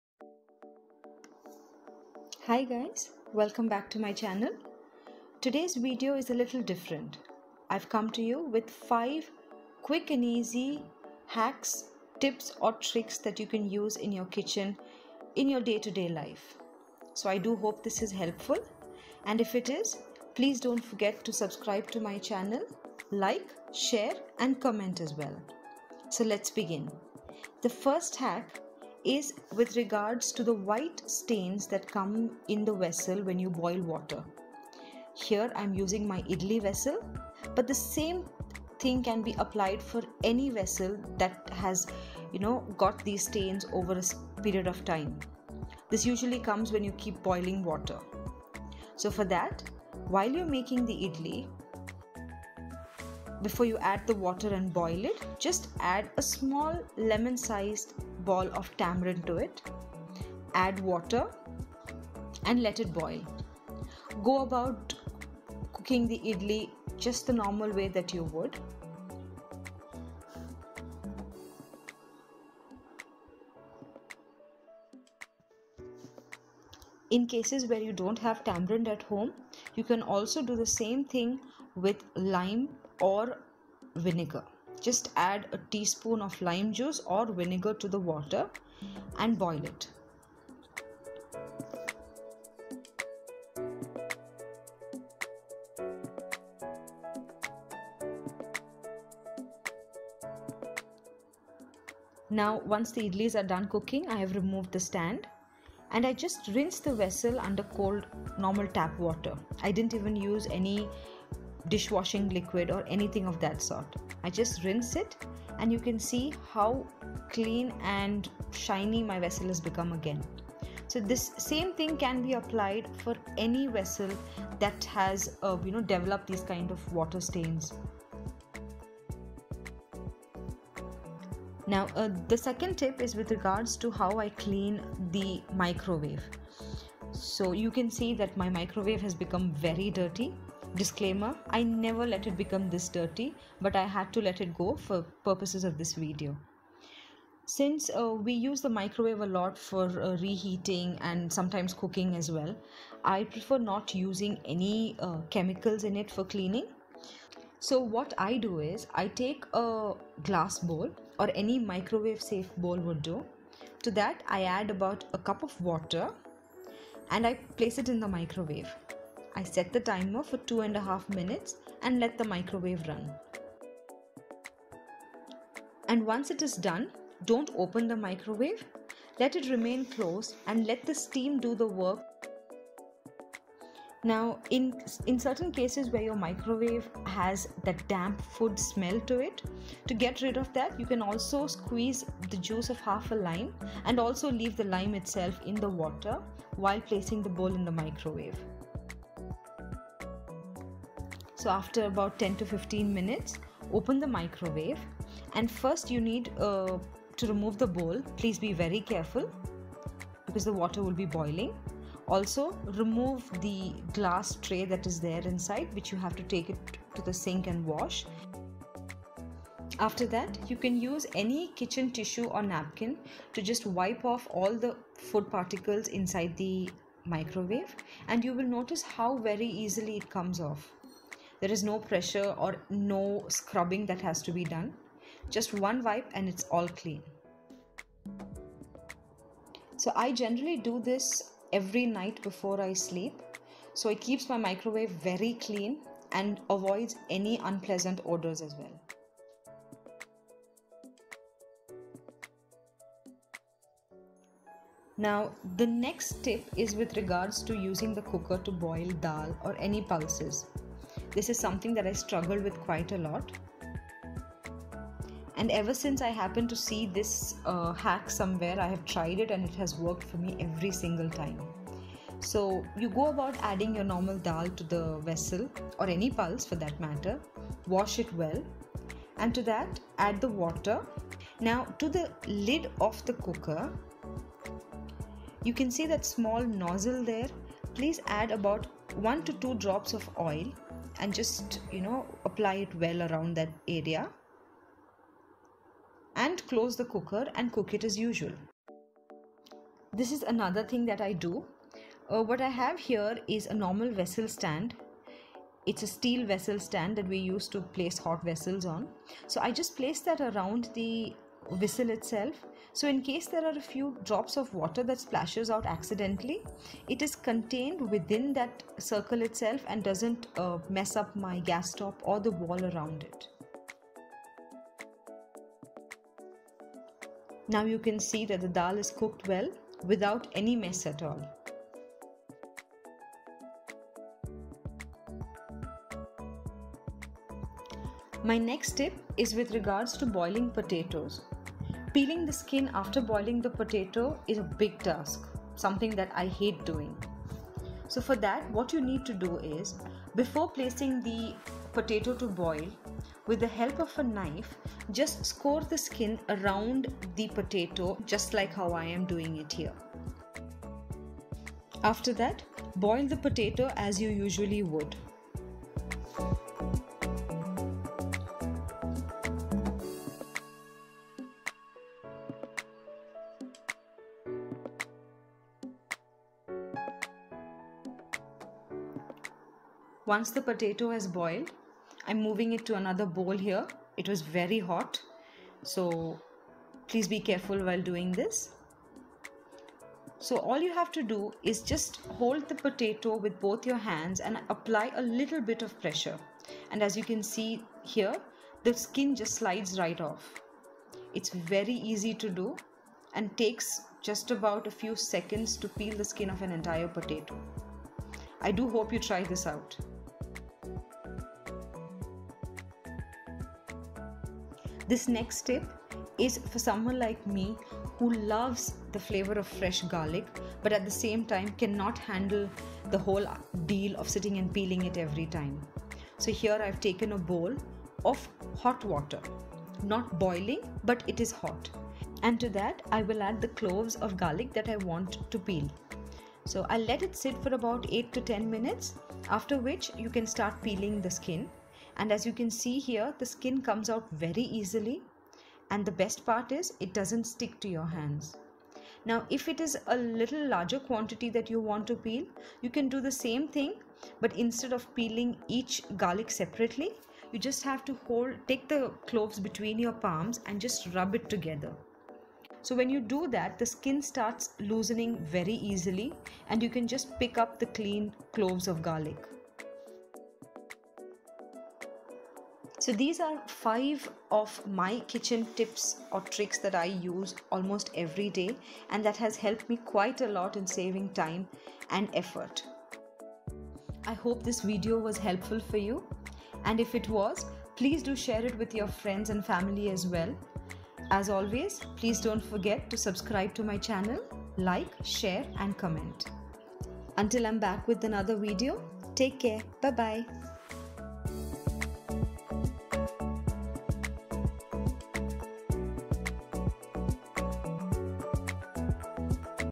5 QUICK AND USEFUL KITCHEN/COOKING TIPS, TRICKS AND HACKS - SMART KITCHEN HACKS
In this video I have shared 5 of my most useful kitchen life hacks. These are everyday tips, trick and hacks that you can apply to your day to day life and save a lot of time and effort.

I do hope you try it out. And share your valuable comments and feedback.

Thanks,
Shimin